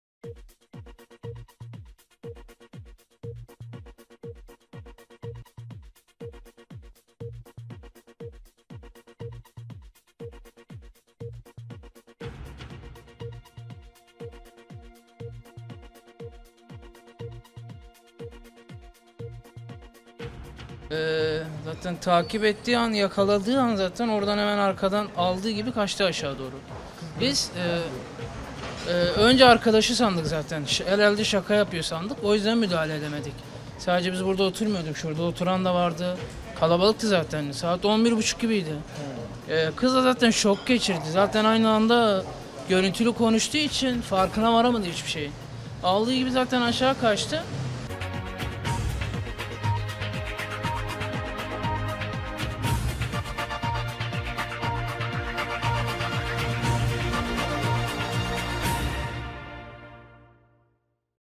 Taksim’de genç kızın yaşadığı kapkaç kamerada
İstanbul’un göbeği Taksim’de sokak arasında yürüdüğü esnada arkadaşıyla görüntülü görüşme yapan genç kız, vatandaşların gözü önünde kapkaça uğradı.